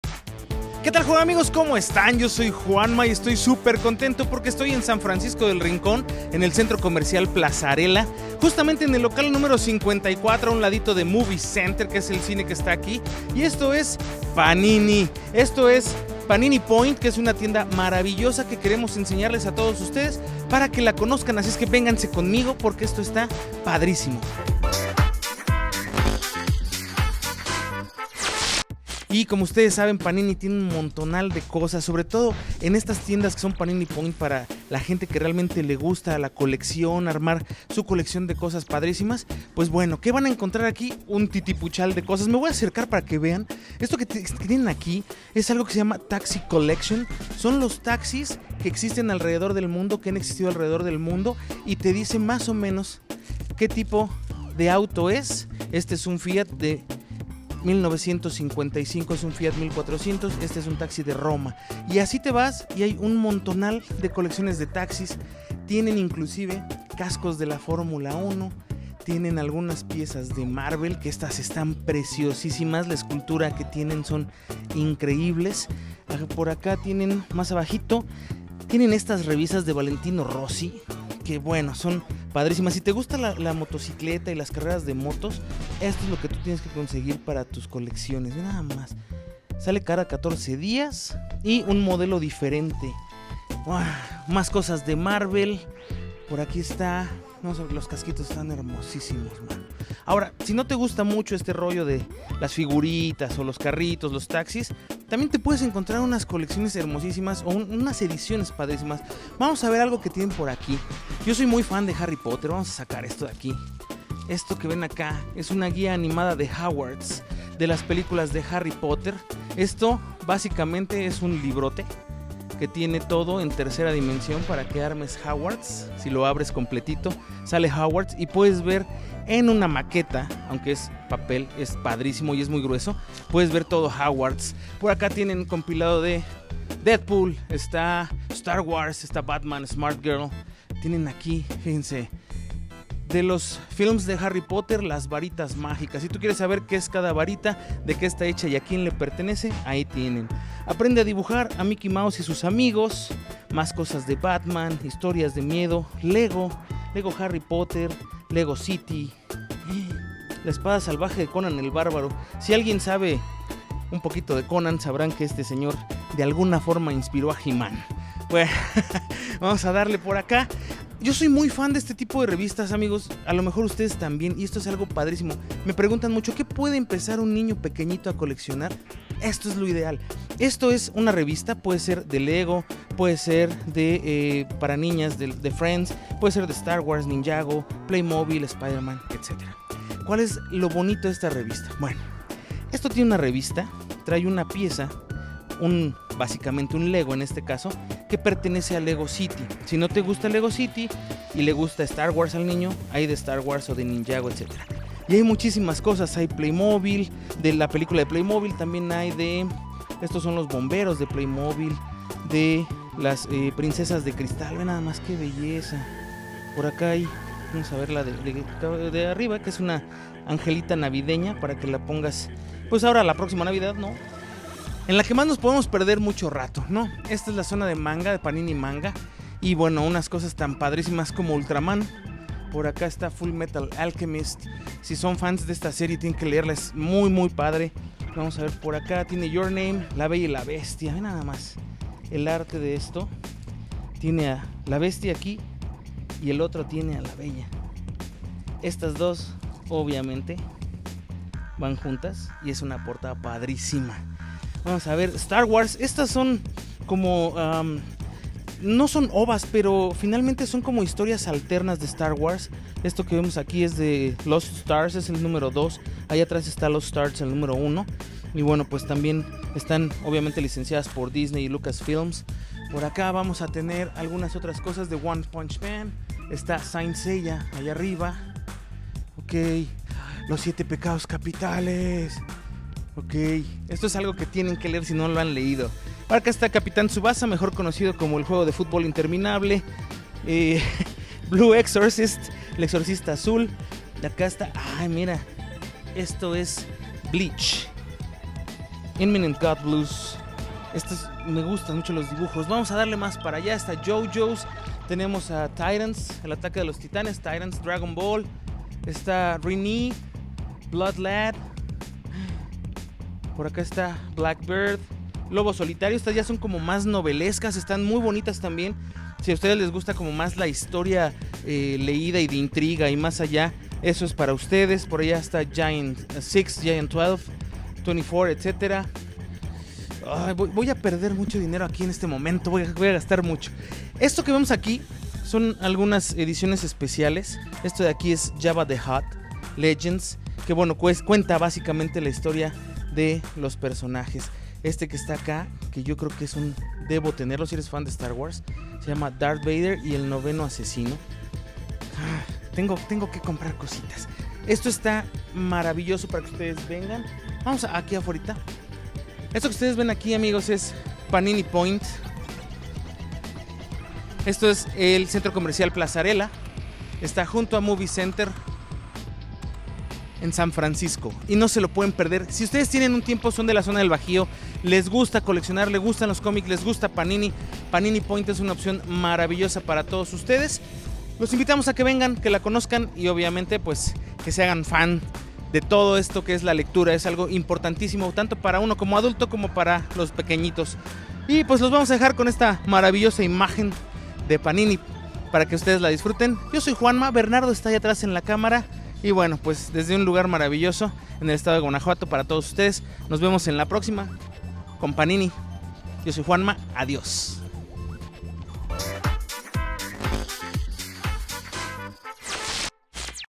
Panini Point San Fco Del Rincón Guanajuato ► Juegos Juguetes y Coleccionables
Recorrido por PaniniPoint San Fco Del Rincón en Guanajuato (tienda panini) estampas, albums, coleccionables, mangas, comics, tpbs y mas